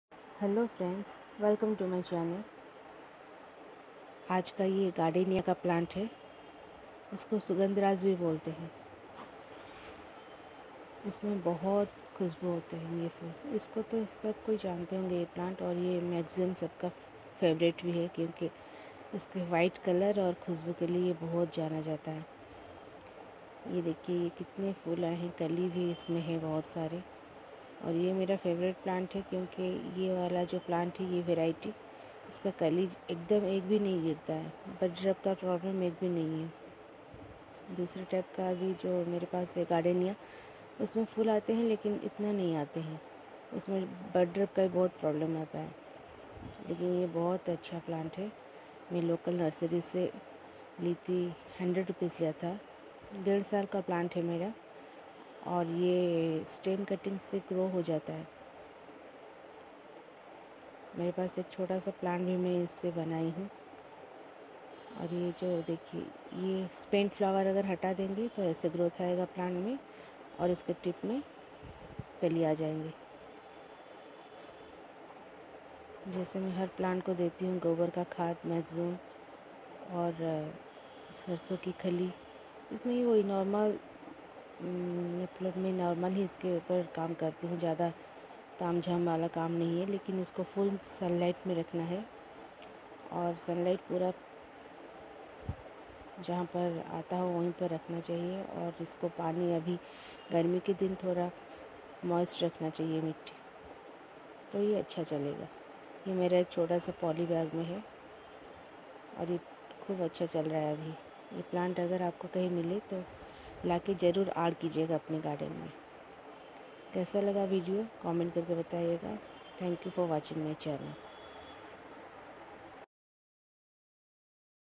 Gardenia flowers
care of gardenia or sugandhraj plant for more flowers